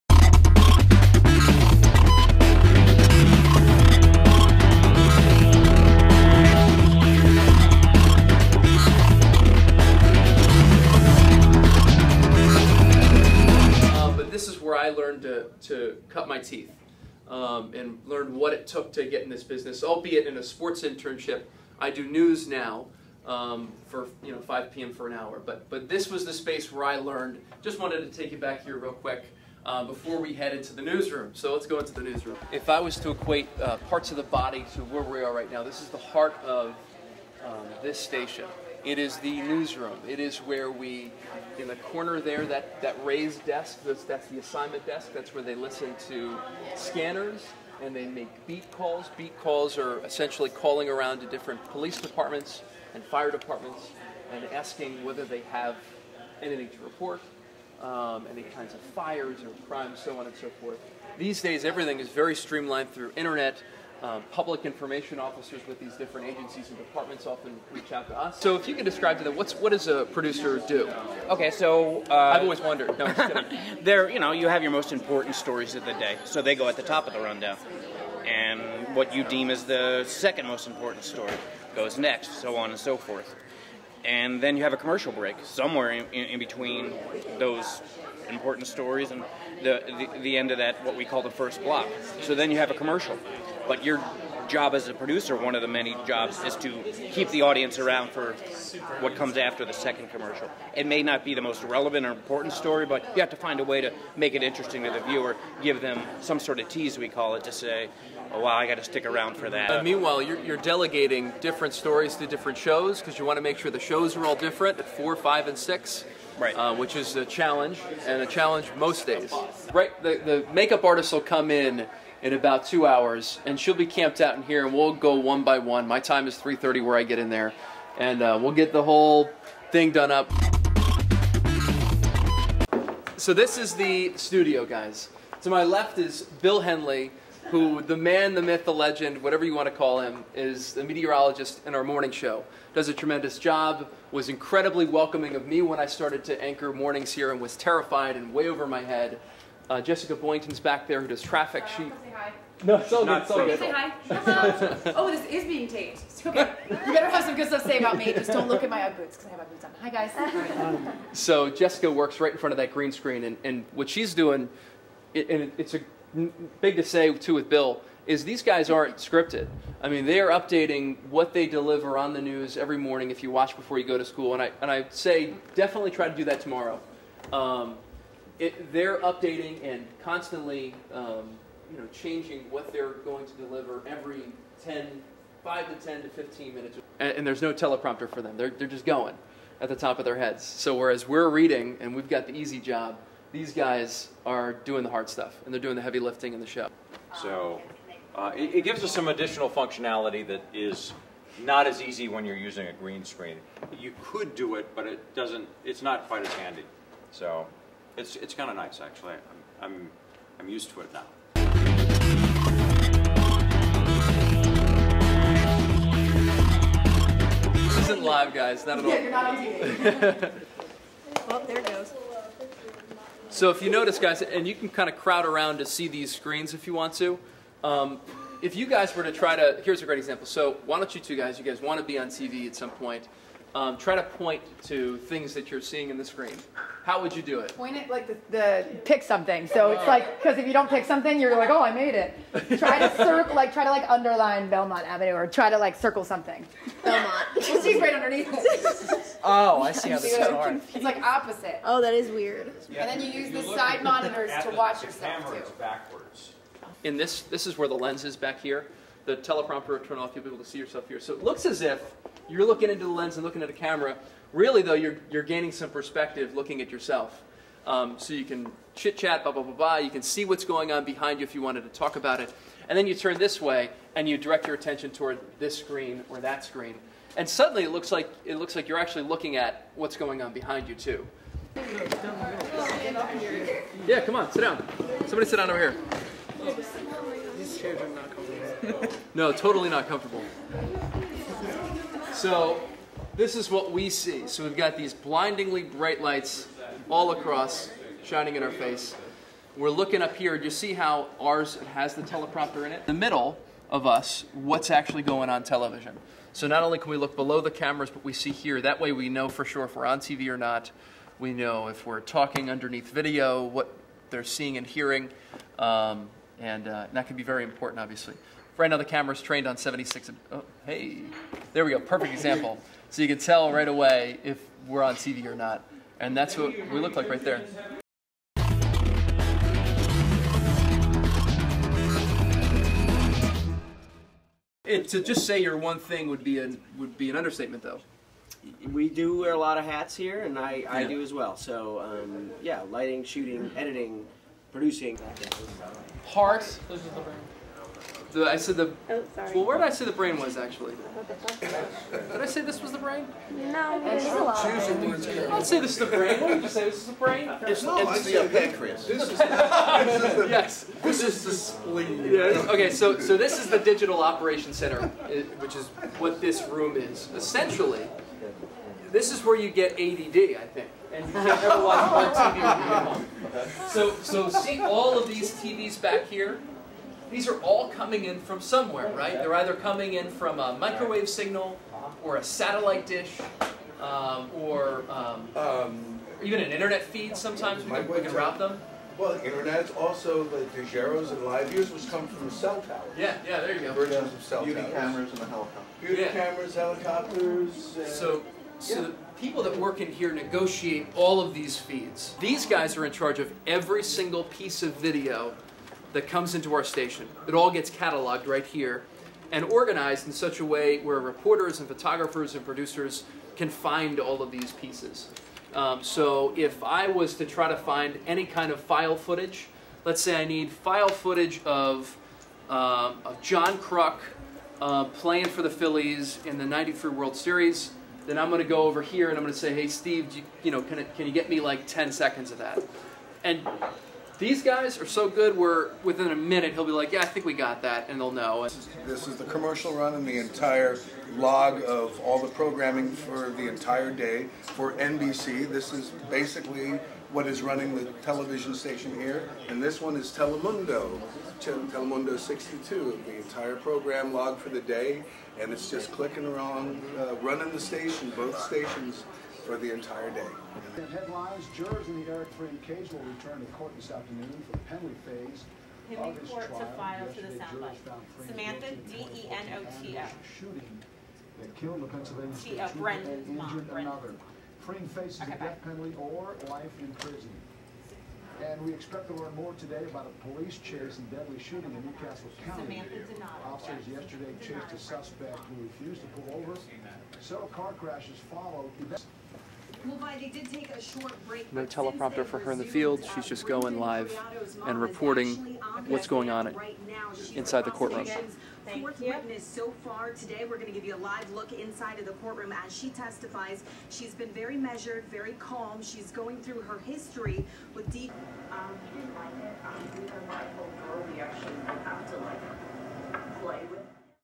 New Egypt HS Mass Media NBC 10 Tour with Keith Jones
Students from New Egypt HS take a tour of NBC 10 with News Anchor Keith Jones